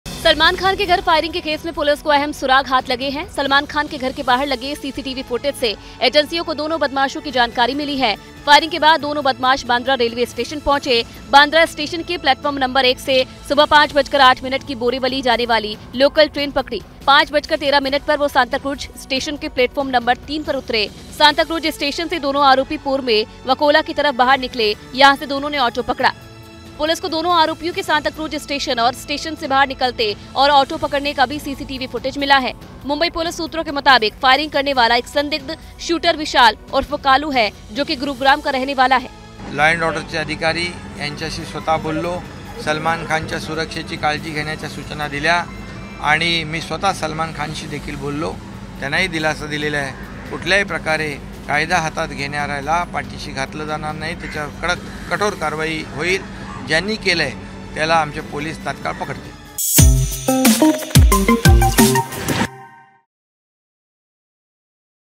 Salman Khan House Attack: सलमान के घर फायरिंग मामले में पुलिस ने दर्ज किया गवाहों के बयान | ABP LIVE
News: सलमान खान के घर के बाहर फायरिंग करनेवाले दोनों बदमाशों की पहचान हो गई है...जांच एजेंसियों को इन बदमाशों के बारे में अहम सुराग भी मिला है...
पुलिस ने अब तक इस मामले में किसी को हिरासत में नहीं लिया है
पुलिस ऑटो रिक्शा ड्राइवर का बयान दर्ज कर रही है
आरोपियों को देखनेवाले दूसरे लोगों के भी बयान लिए जा रहे हैं